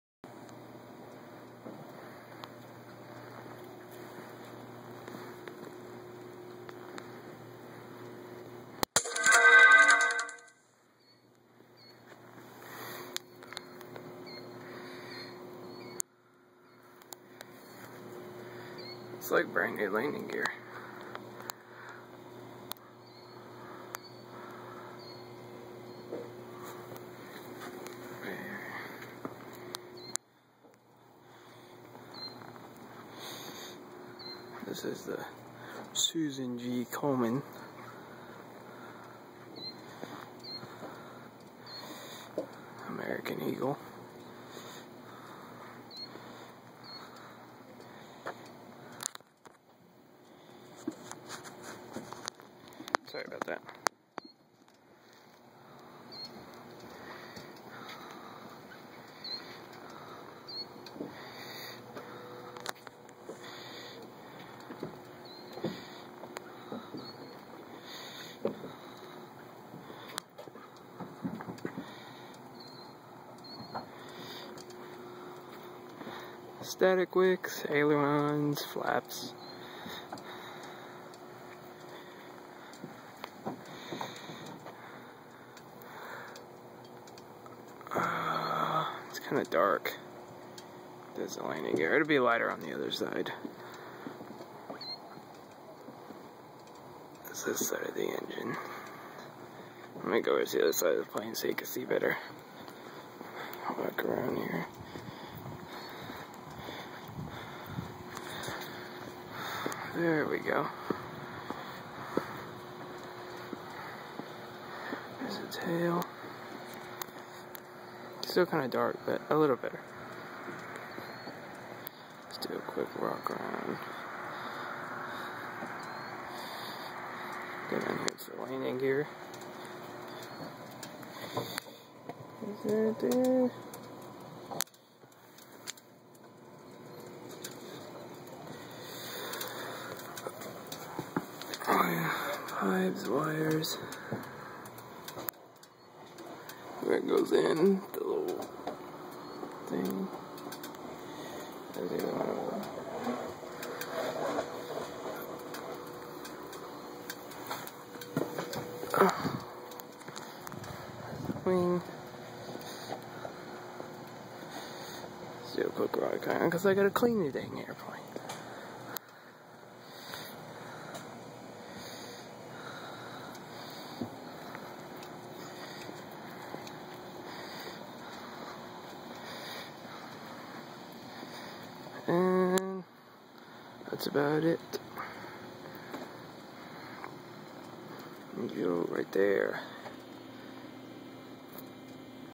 american eagle embraer erj-145 walkaround
taken at tyler pounds regional airport aircraft N640AE